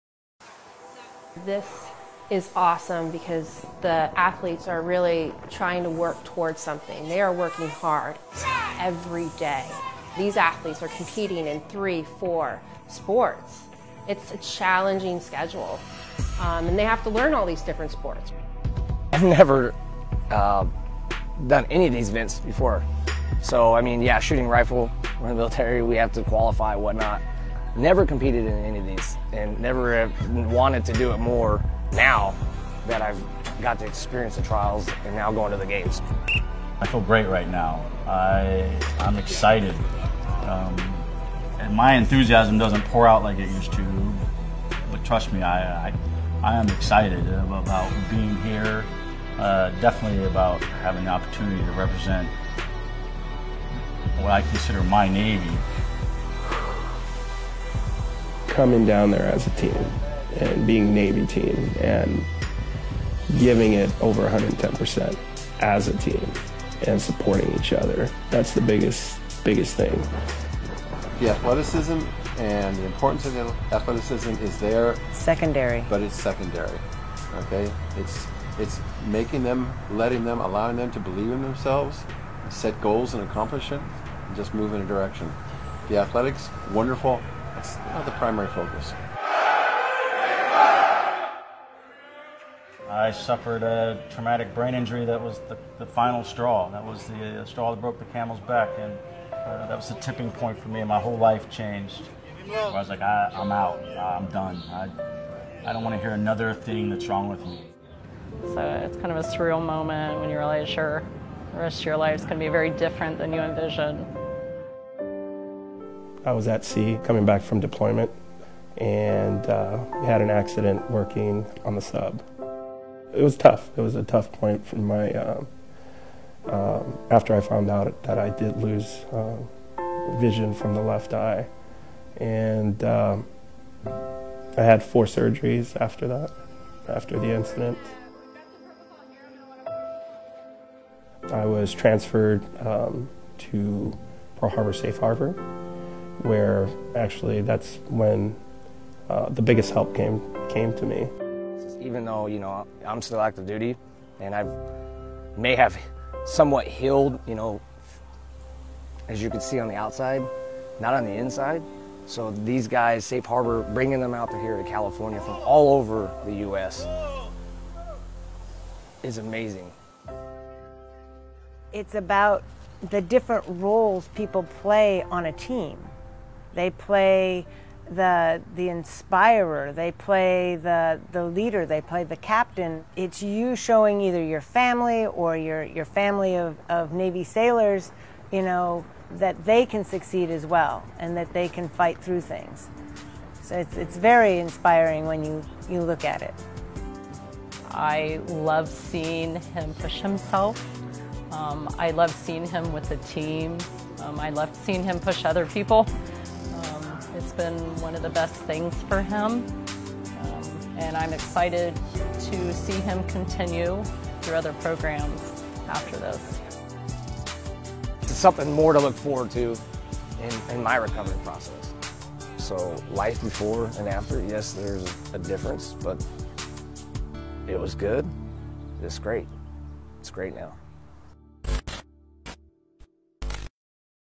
Safe Harbor
As they prepare for the Warrior Games, members of the U.S. Navy team talk about what competing in the games means to them.